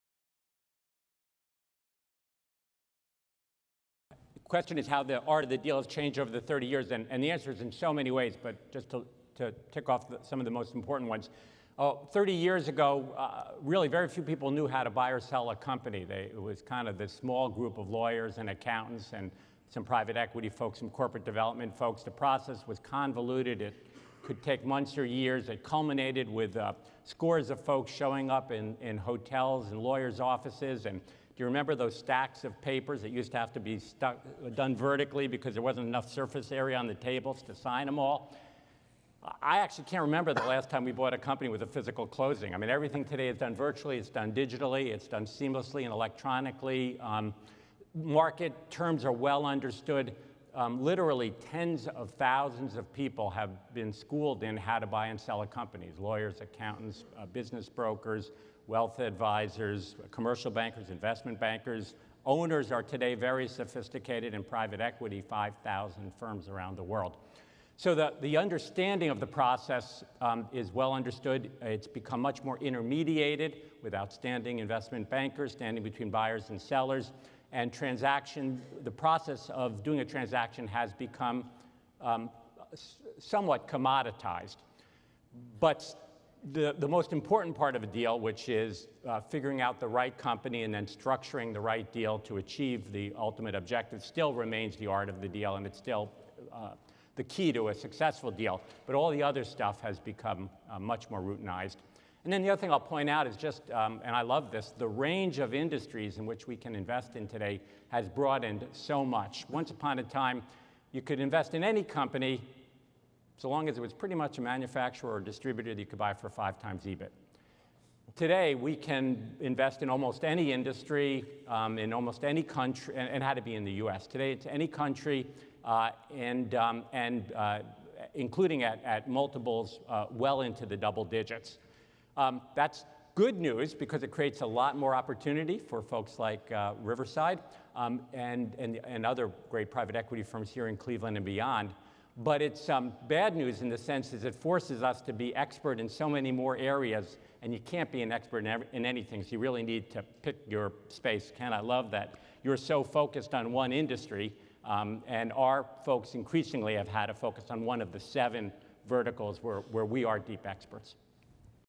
Stewart Kohl on How the Art of the Deal has Changed
Stewart Kohl, co-CEO of The Riverside Co., shared how the art of the deal has changed over the last 30 years with the Aspire 2017 audience. Stewart is an Honorary Chair of the event and took part in the Entrepreneurs’ M&A Roundtable.